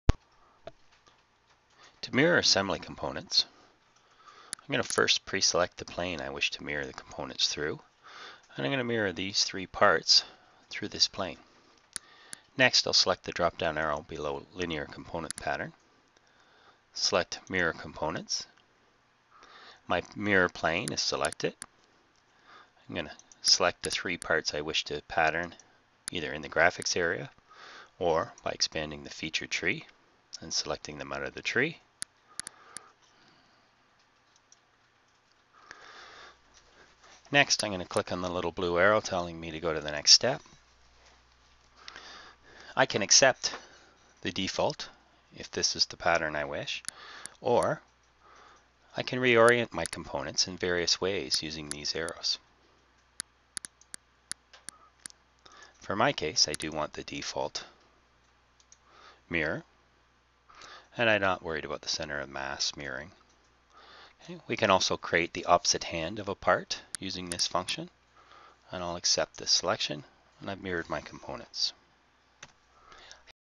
Mirror Assembly Components
How to mirror components in a SolidWorks assembly.